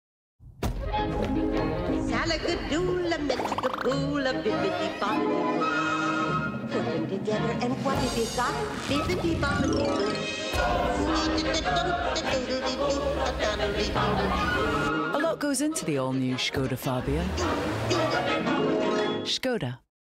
ŠKODA FABIA - Telford ŠKODA Carlisle
The all new ŠKODA FABIA available to test drive at Telfords ŠKODA Carlisle.